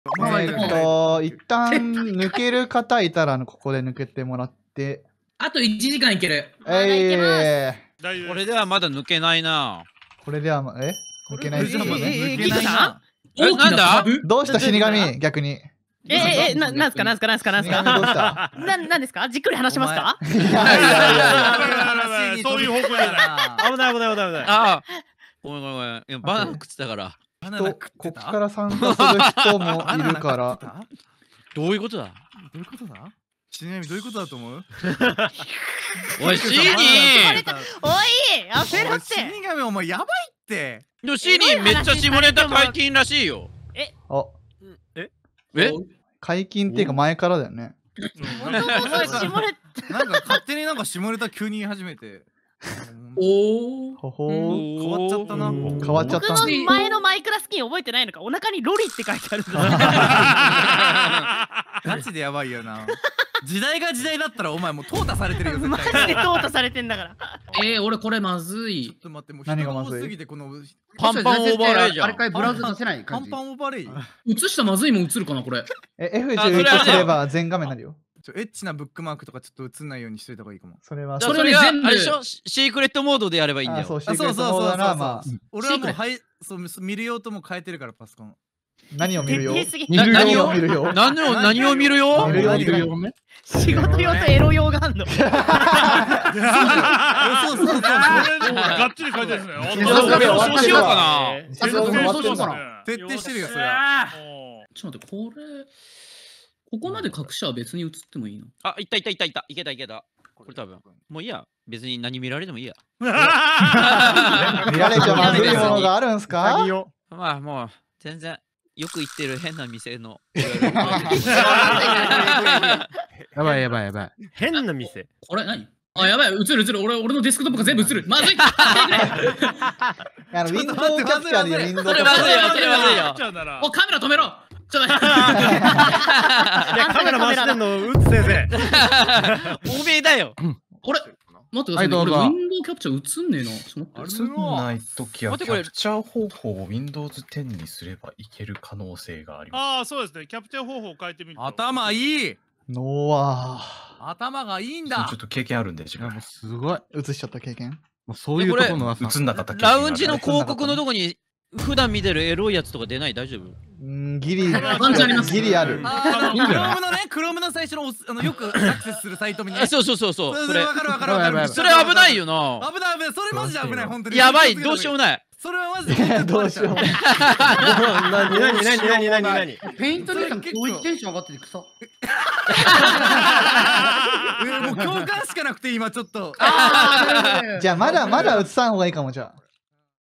【リビング注意】しにー下ネタ解禁らしいよ!!下ネタが止まらない男たち【らっだぁ切り抜き】
※らっだぁ公認の切り抜きチャンネルです※